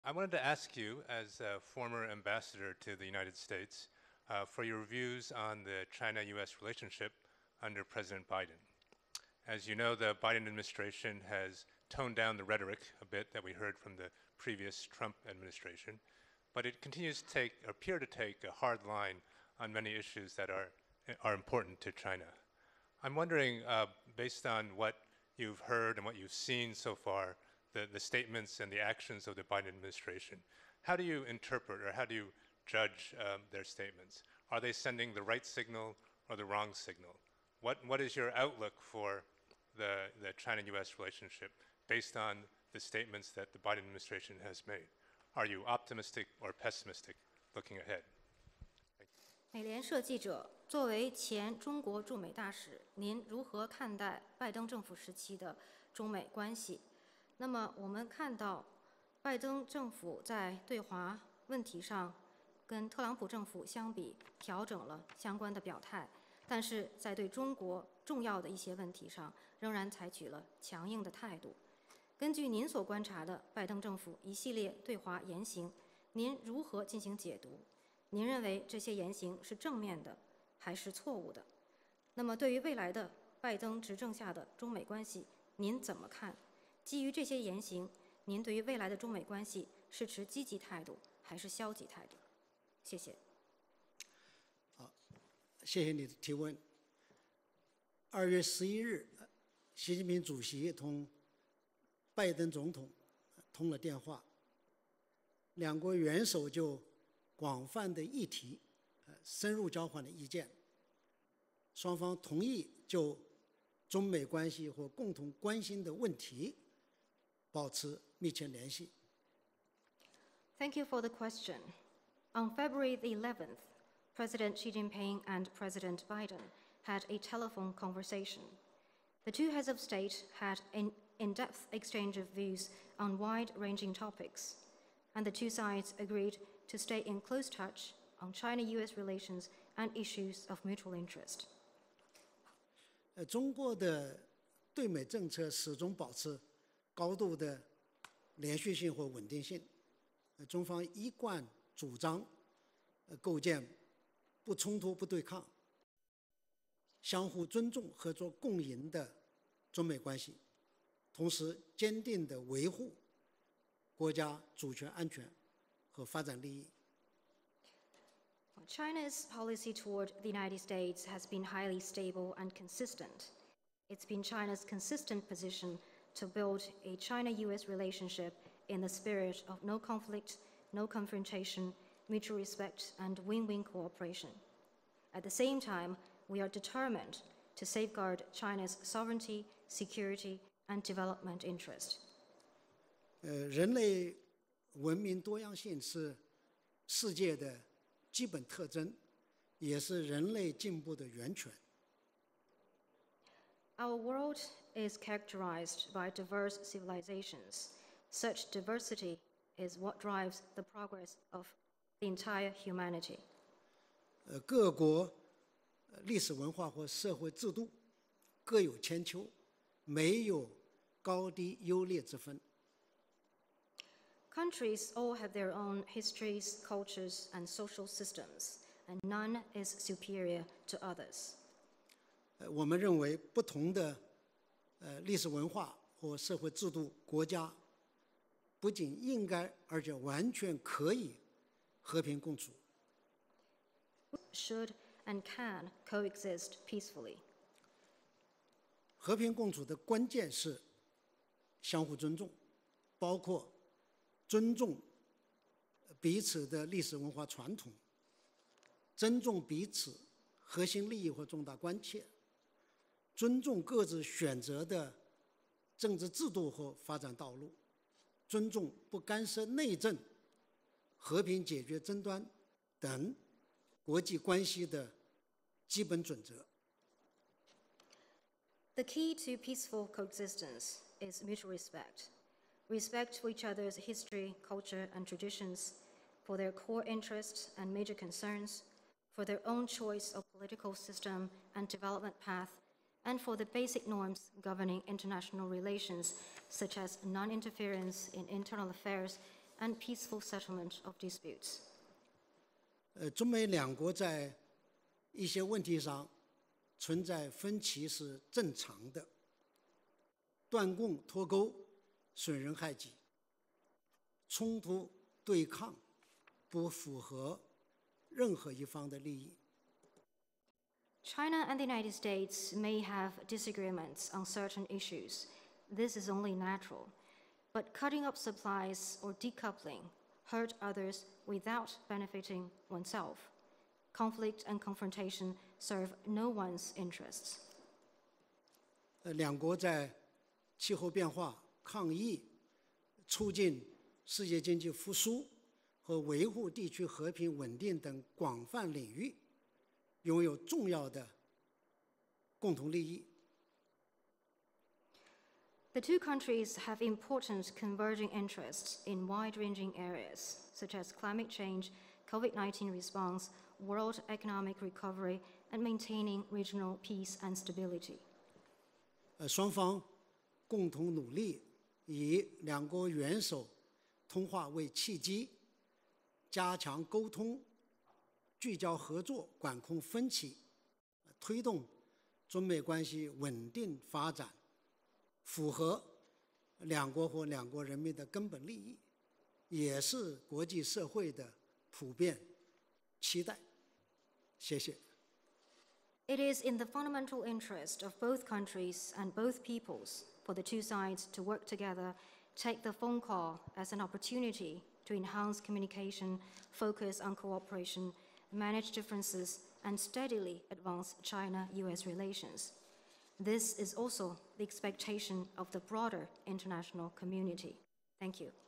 Mutual respect, win-win cooperation important for China-U.S. ties: NPC spokesperson
Zhang Yesui, spokesperson for the fourth session of the 13th National People's Congress (NPC), said that China always stands for building a China-U.S. relationship featuring no conflict, no confrontation, mutual respect and win-win cooperation while firmly safeguarding national sovereignty, security and development interests.

He made the remarks during Thursday's press conference when answering a reporter's question on China-U.S. ties during U.S. President Joe Biden's administration.